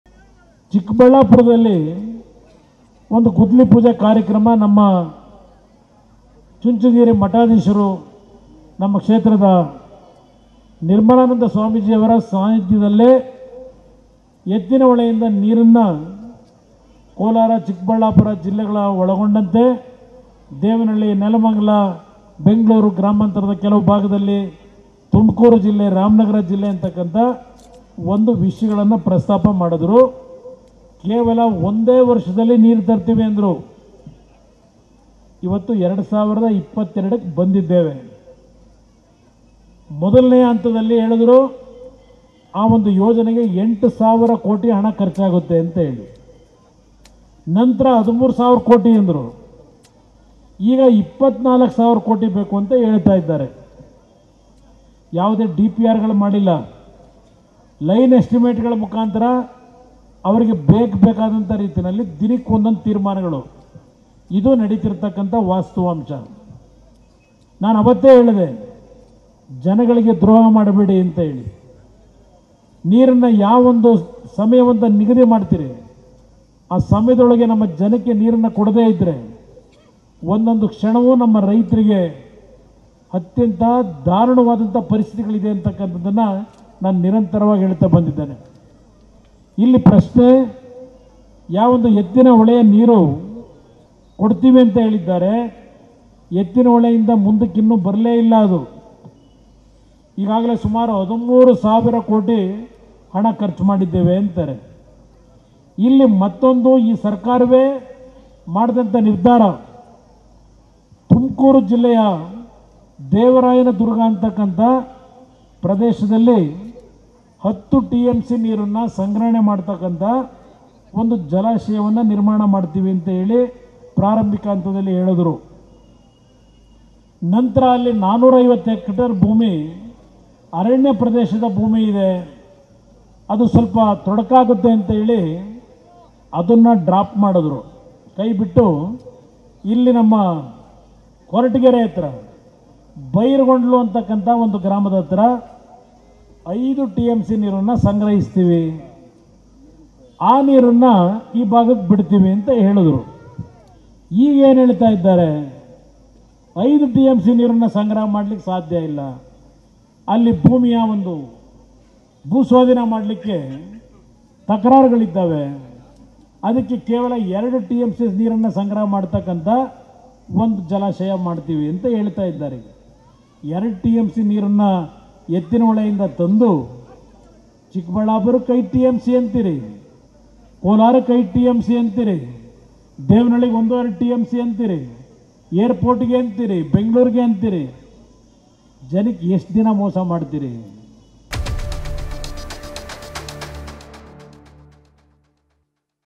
HD Kumaraswamy : ನೀರಾವರಿ ವಿಚಾರವಾಗಿ ಎಷ್ಟುದಿನ ಮೋಸ ಮಾಡ್ತೀರಿ
HD Kumaraswamy : ನೀರಾವರಿ ವಿಚಾರವಾಗಿ ಎಷ್ಟುದಿನ ಮೋಸ ಮಾಡ್ತೀರಿ ರಾಷ್ಟ್ರೀಯ ಪಕ್ಷಗಳಿಗೆ ಚಾಟಿ ಬೀಸಿದ HDK |